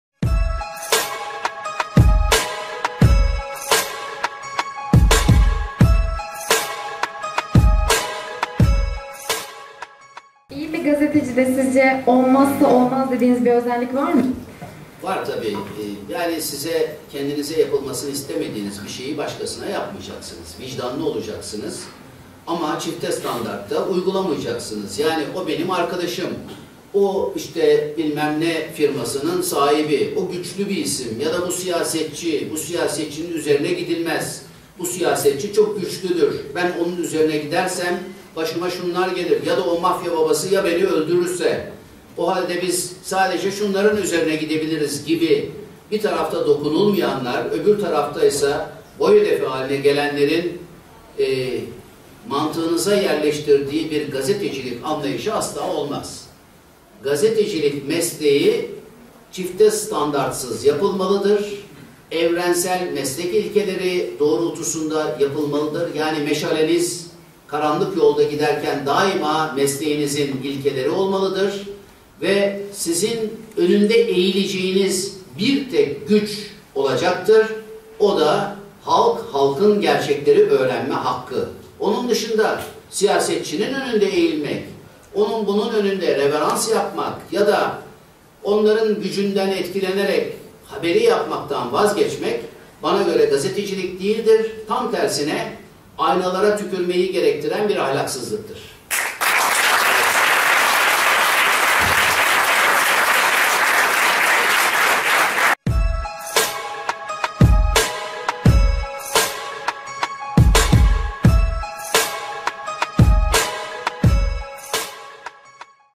Gazeteci nasıl davranmalıdır
Uğur Dündar anlatıyor: Gazeteci nasıl davranmalıdır